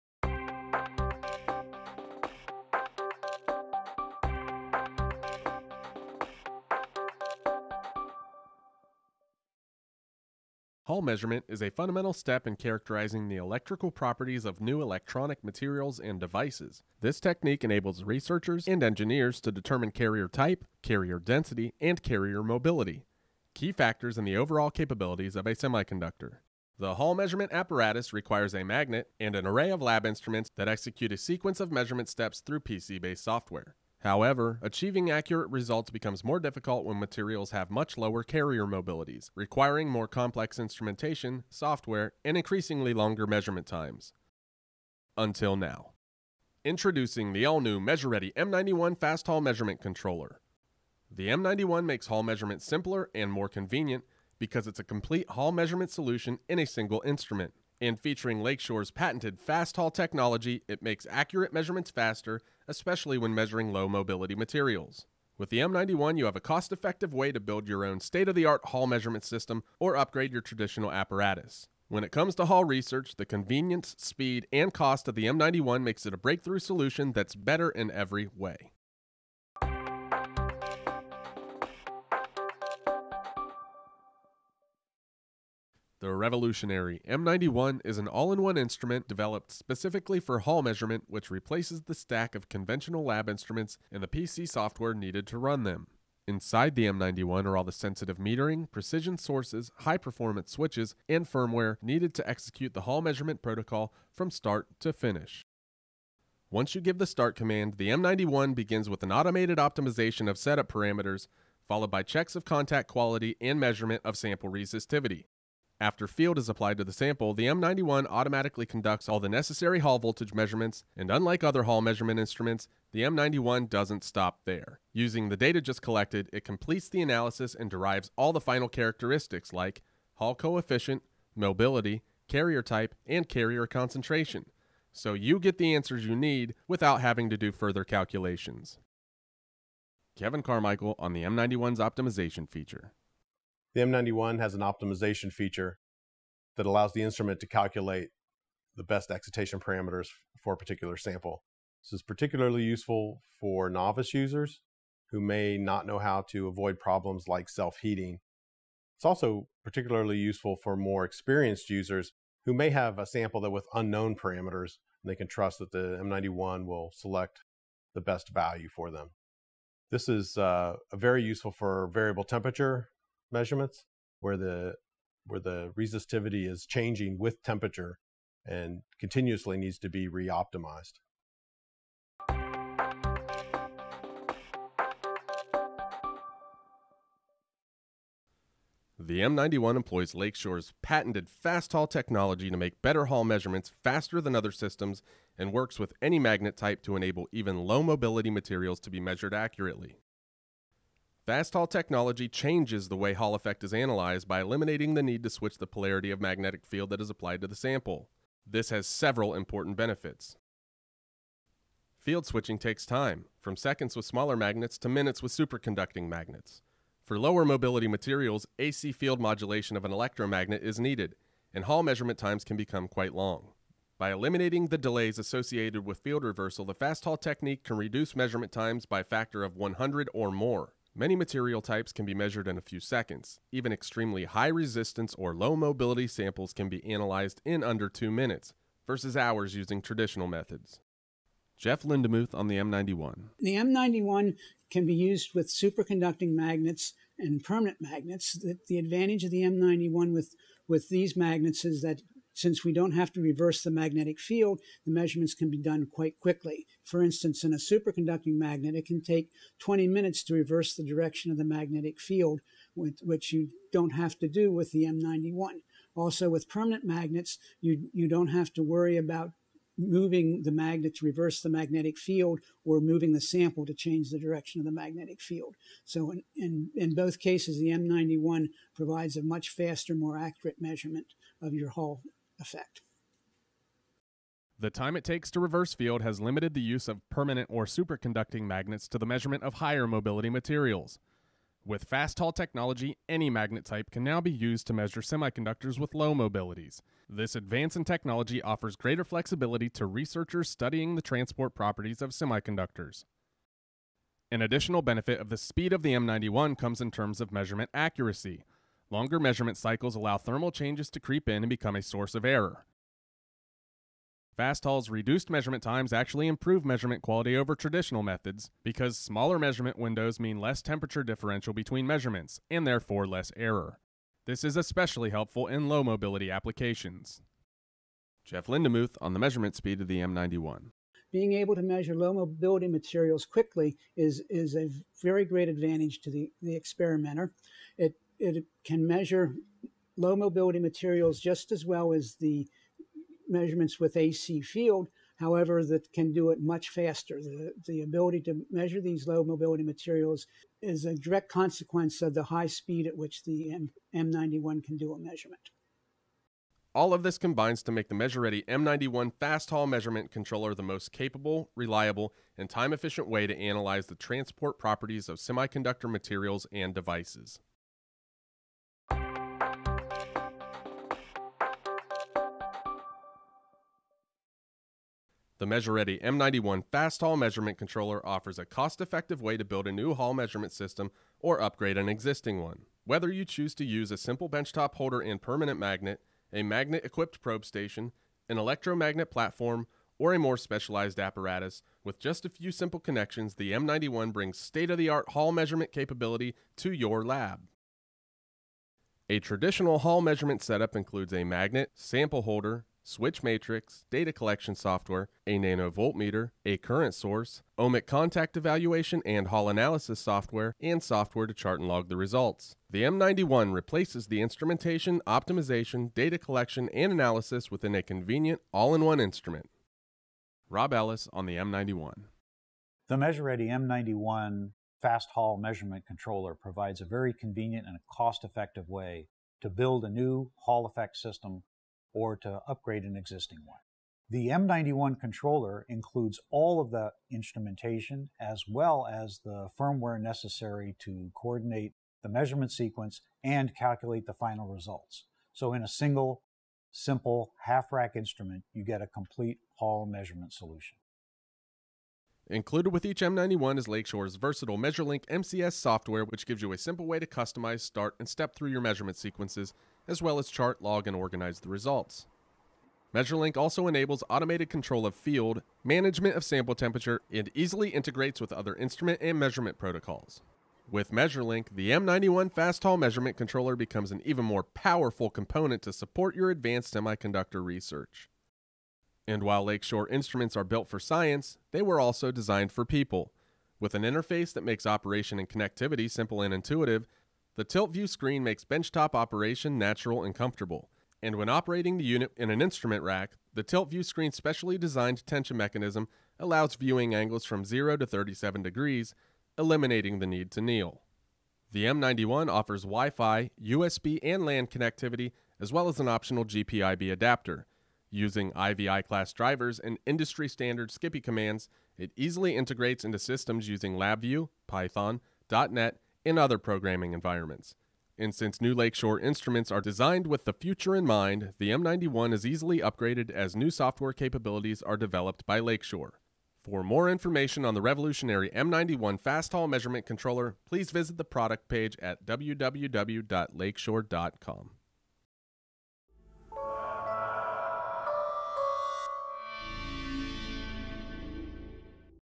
MeasureReady M91 FastHall™ Measurement Controller: First Look
Featuring Lake Shore’s patented new FastHall measurement technique, the M91 fundamentally changes the way the Hall effect is measured by eliminating the need to switch the polarity of the applied magnetic field during the measurement. This breakthrough results in faster and more accurate measurements, especially when using high-field superconducting magnets or when measuring very low-mobility materials.